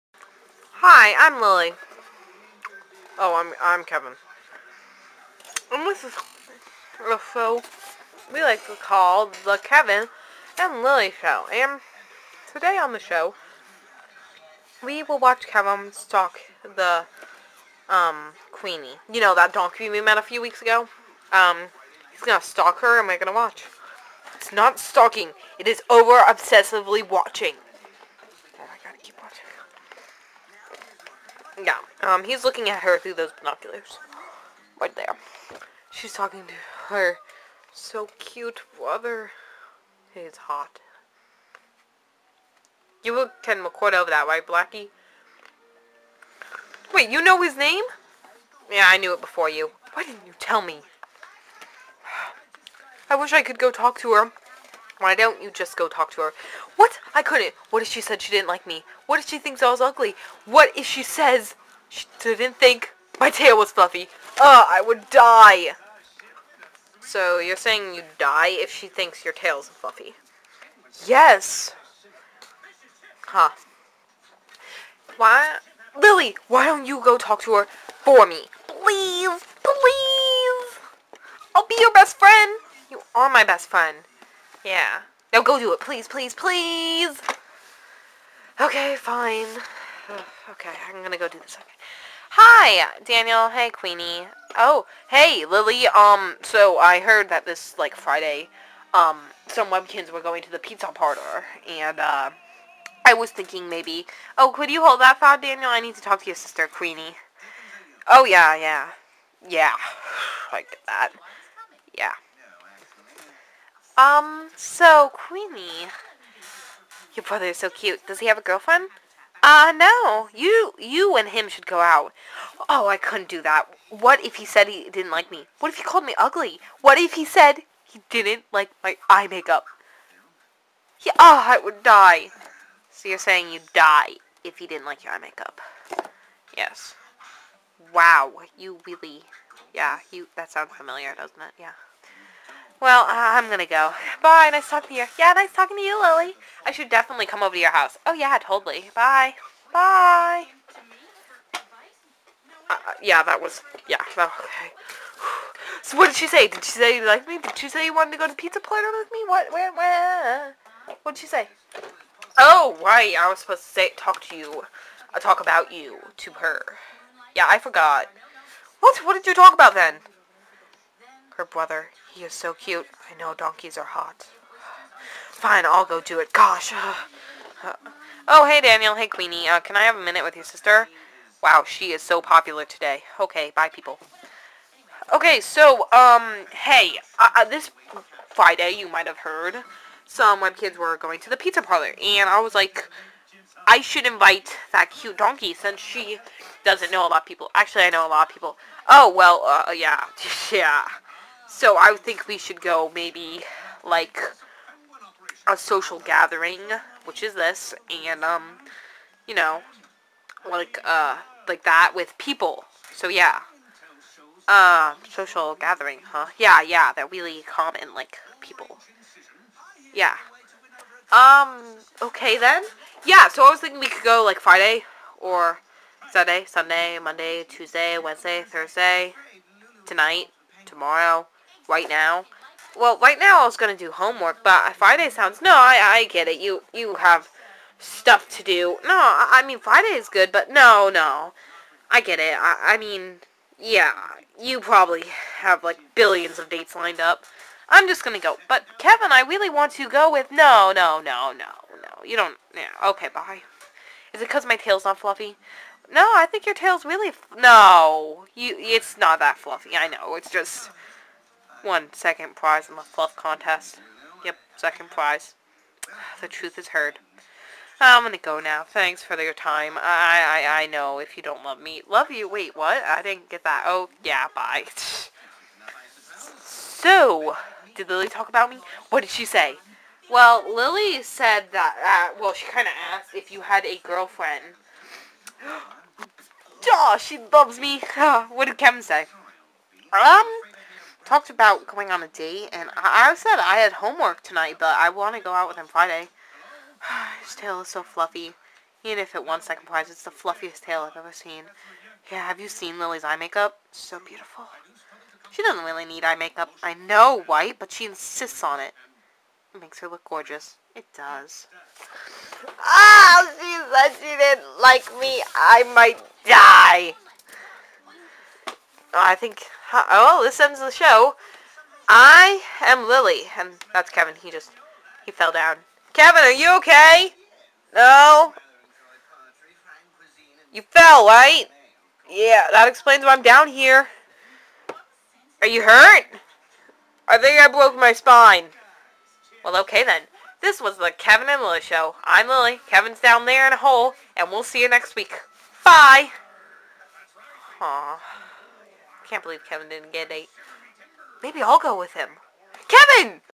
The Keven and Lilly Show Ep. 10
Today Keven "Over obsessively watches" Queeny.